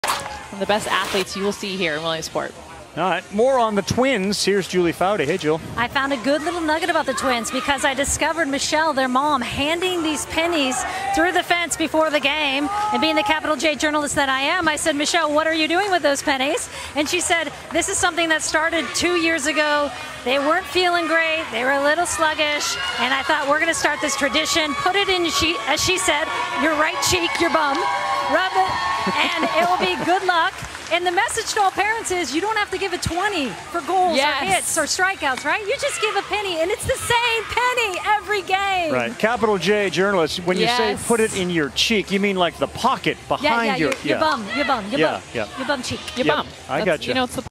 2021 LLWS
Mini story of the Lucier twins from New Hampshire